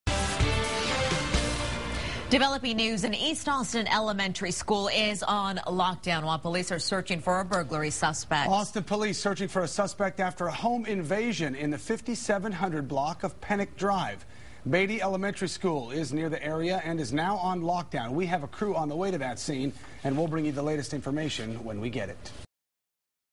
Police lift lockdown on Baty Elementary
Police are looking for a suspected burglar after a reporter home invasion in the area.